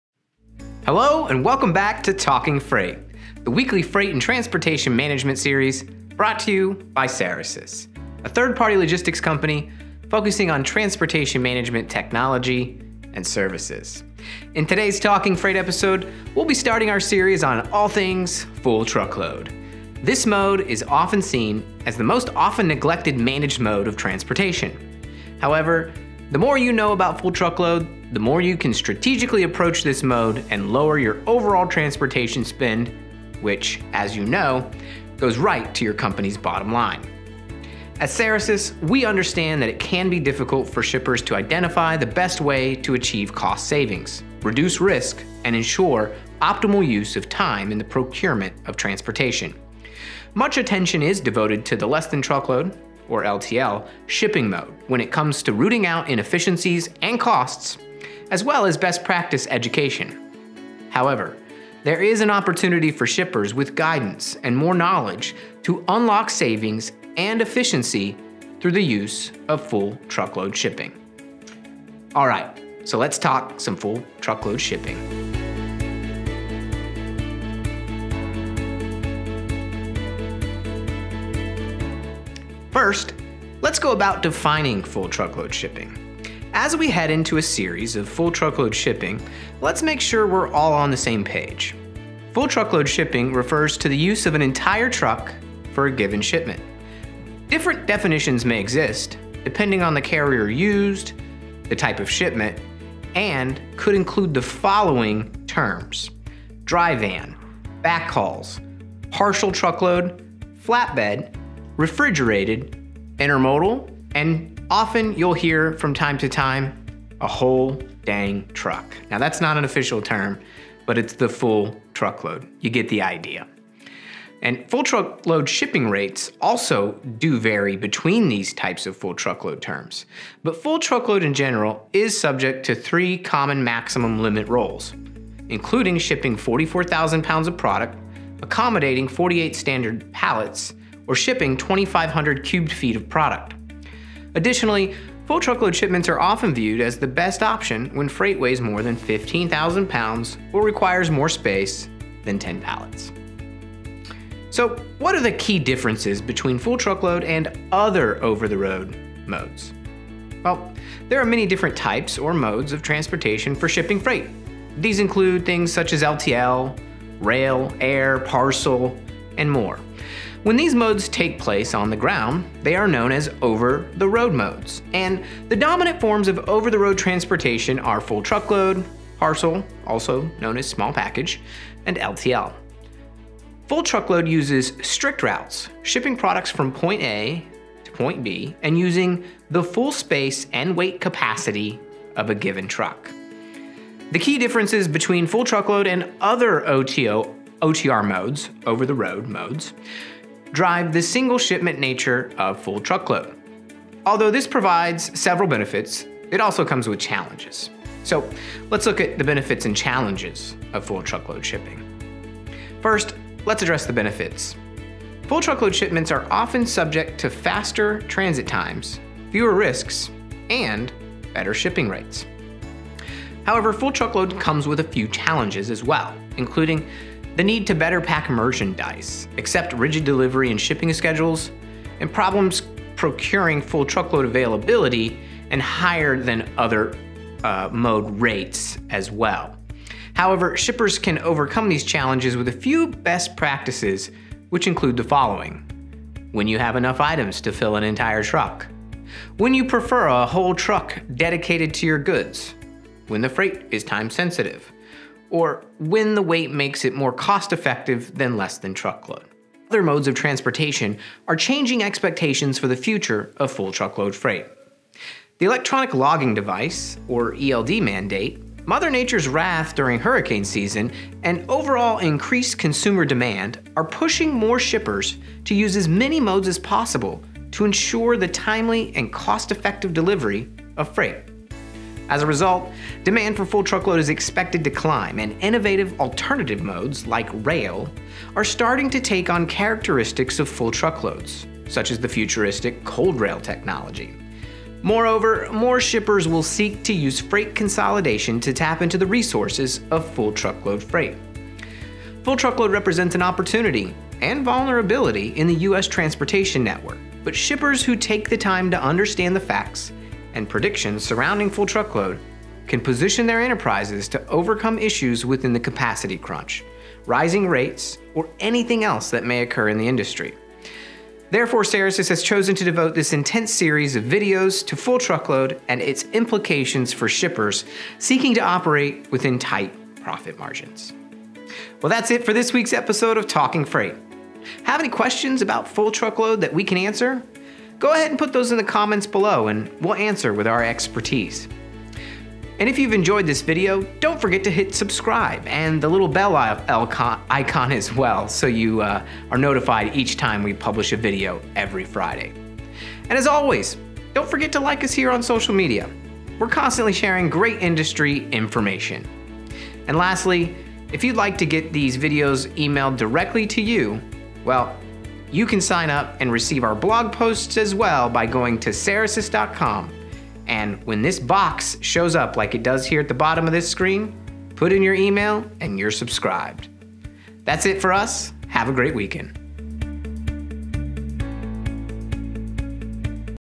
Talking Freight (49) - What Is Full Truckload Shipping & How It Differs From Other OTR Modes?
Did you know full truckload shipping offers many benefits that make it stand apart from other transportation modes and benefit shippers? Learn more now in this week's episode of "Talking Freight."